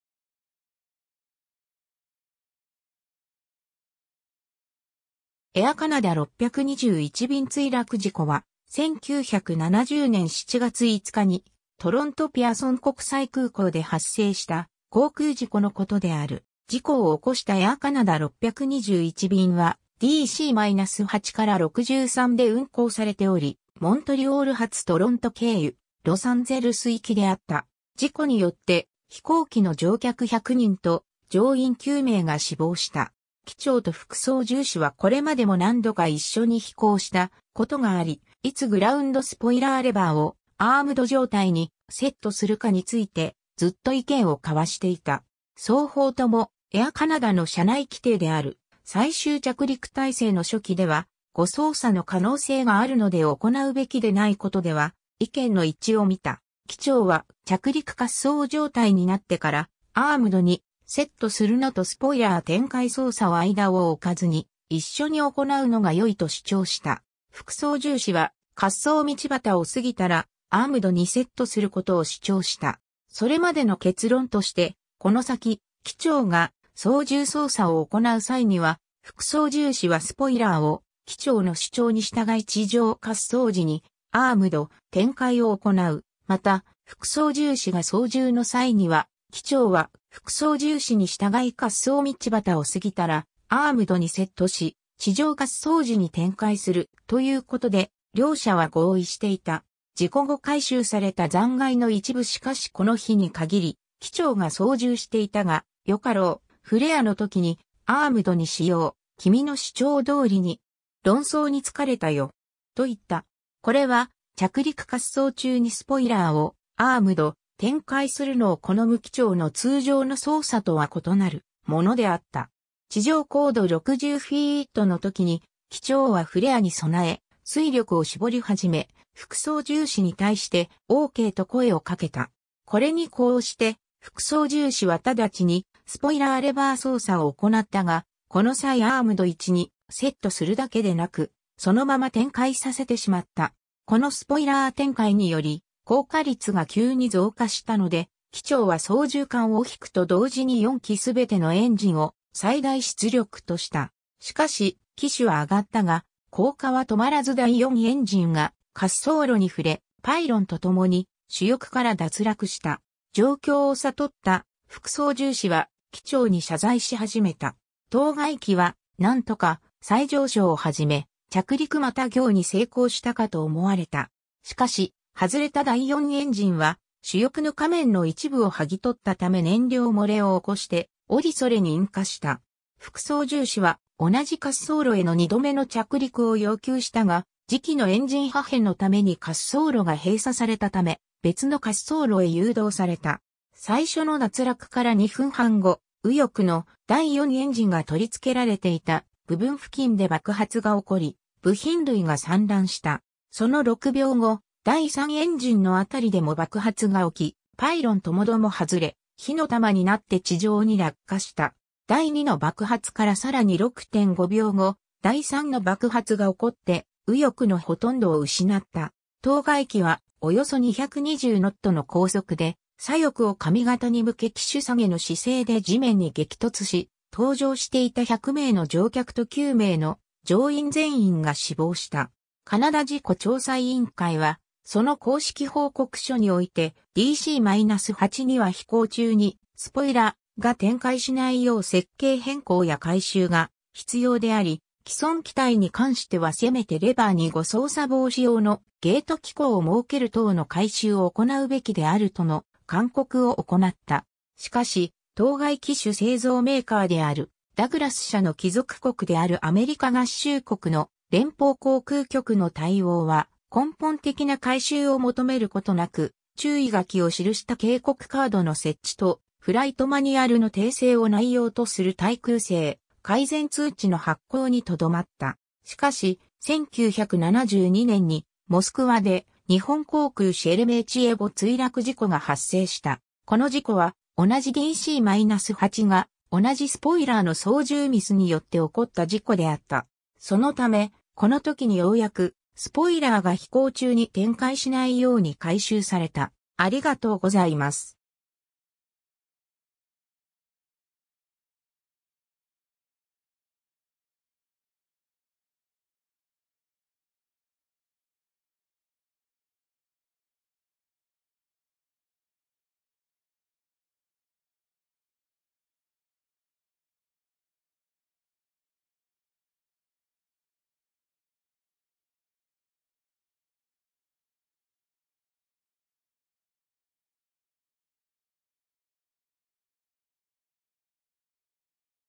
エア・カナダ621便墜落事故
エア・カナダの航空事故
エア・カナダ621便墜落事故（エア・カナダ621びんついらくじこ）は、1970年7月5日にトロント・ピアソン国際空港で発生した航空事故のことである。
 事故を起こしたエア・カナダ621便は、DC-8-63（機体記号CF-TIW）で運航されており、モントリオール発トロント経由ロサンゼルス行きであった。
事故によって、飛行機の乗客100人と乗員9名が死亡した。
 機長と副操縦士はこれまでも何度か一緒に飛行したことがあり、いつグラウンドスポイラーレバーをアームド状態にセットするかについてずっと意見を交わしていた。
双方とも、エア・カナダの社内規定である「最終着陸態勢（ファイナルアプローチ）の初期」では誤操作の可能性があるので行うべきでないことでは意見の一致を見た。
機長は、着陸滑走状態になってから、アームドにセットするのとスポイラー展開操作を間をおかずに一緒に行うのが良いと主張した。
副操縦士は、滑走路端を過ぎたらアームドにセットすることを主張した。
 それまでの結論として、この先、機長が操縦操作を行う際には副操縦士はスポイラーを機長の主張に従い地上滑走時にアームド/展開を行う、また、副操縦士が操縦の際には機長は副操縦士に従い滑走路端を過ぎたらアームドにセットし、地上滑走時に展開する、ということで両者は合意していた。
 事故後回収された残骸の一部 しかしこの日に限り、機長が操縦していたが「よかろう、フレアのときにアームドにしよう、君の主張どおりに。論争に疲れたよ。」と言った。
これは、着陸滑走中にスポイラーをアームド / 展開するのを好む機長の通常の操作とは異なるものであった。
地上高度60フィートの時に、機長はフレアに備え、推力を絞り始め、副操縦士に対して「OK」と声を掛けた。
これに呼応して副操縦士は直ちにスポイラーレバー操作を行ったが、この際アームド位置にセットするだけでなく、そのまま展開させてしまった。
このスポイラー展開により降下率が急に増加したので、機長は操縦桿を引くと同時に4基すべてのエンジンを最大出力とした。
しかし、機首は上がったが降下は止まらず（スロットル操作を行っても実際に出力が最大になるには数秒かかる）第4エンジンが滑走路に触れ、パイロンとともに主翼から脱落した。
状況を悟った副操縦士は、機長に謝罪し始めた。
当該機は何とか再上昇を始め、着陸復行に成功したかと思われた。
しかし、外れた第4エンジンは、主翼の下面の一部をはぎ取ったため燃料漏れを起こしておりそれに引火した。
副操縦士は同じ滑走路への2度目の着陸を要求したが、自機のエンジン破片のために滑走路が閉鎖されたため別の滑走路へ誘導された。
 最初の脱落から2分半後、右翼の第4エンジンが取り付けられていた部分付近で爆発が起こり、部品類が散乱した。
その6秒後、第3エンジンのあたりでも爆発が起き、パイロンともども外れ、火の玉になって地上に落下した。
第2の爆発からさらに6.5秒後、第3の爆発が起こって右翼のほとんどを失った。
当該機は、およそ 220 ノットの高速で左翼を上方に向け機首下げの姿勢で地面に激突し、搭乗していた100名の乗客と9名の乗員全員が死亡した。
 カナダ事故調査委員会はその公式報告書において、DC-8には飛行中にスポイラ―が展開しないよう設計変更や改修が必要であり、既存機体に関してはせめてレバーに誤操作防止用のゲート機構を設ける等の改修を行うべきであるとの勧告を行った。
しかし、当該機種製造メーカーであるダグラス社の帰属国であるアメリカ合衆国の連邦航空局 (FAA) の対応は、根本的な改修を求めることなく、注意書きを記した警告カードの設置とフライトマニュアルの訂正を内容とする耐空性改善通知の発行にとどまった。
 しかし、1972年にモスクワで日本航空シェレメーチエヴォ墜落事故が発生した。
この事故は同じDC-8が、同じスポイラーの操縦ミスによって起こった事故であった。
そのため、この時にようやく、スポイラーが飛行中に展開しないように改修された。